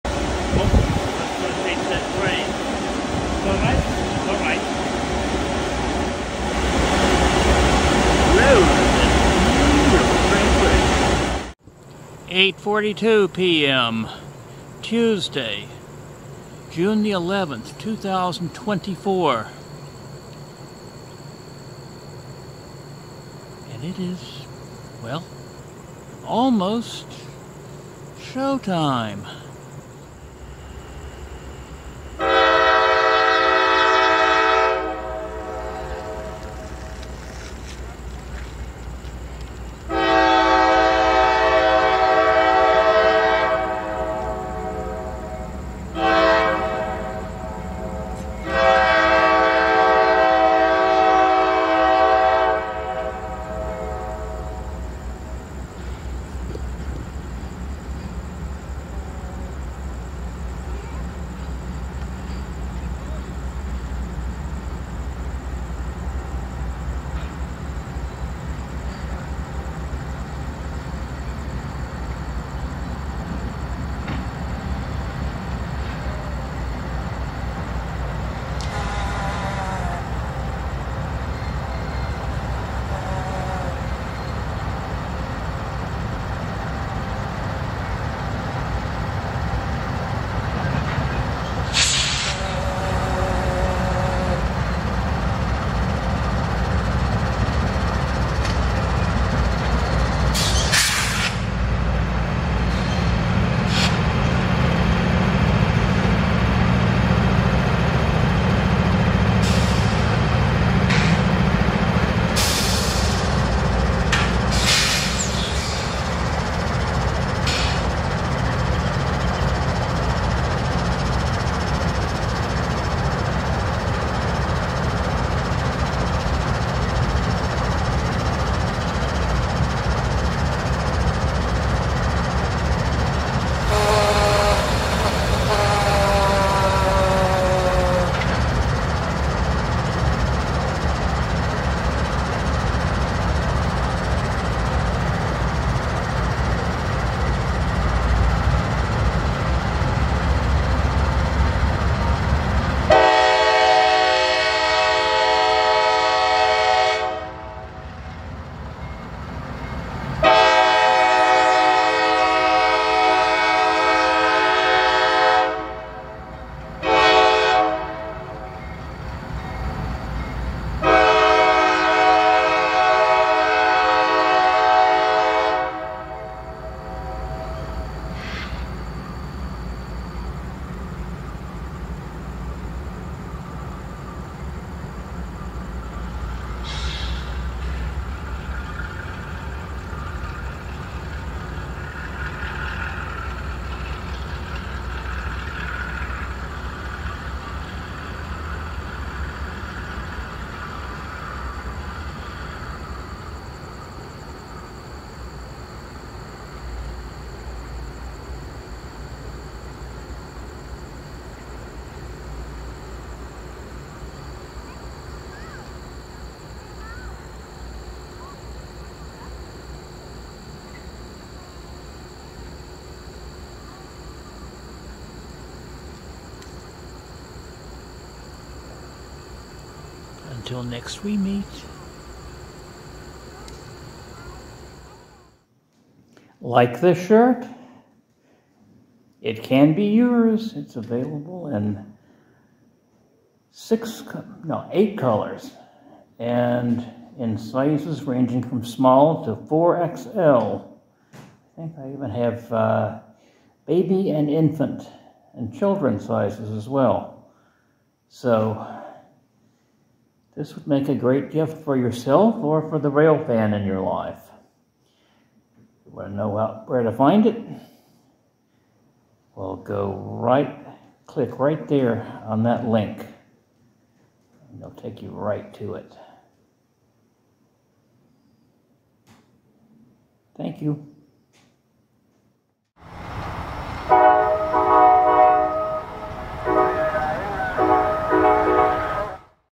S04E164 Turning Three Diesels
Join me trackside at Devine Junction on a nice June evening as we watch a trio of units getting wyed, most likely for service on the following morning's 12R. Please click the link, enjoy, and share freely.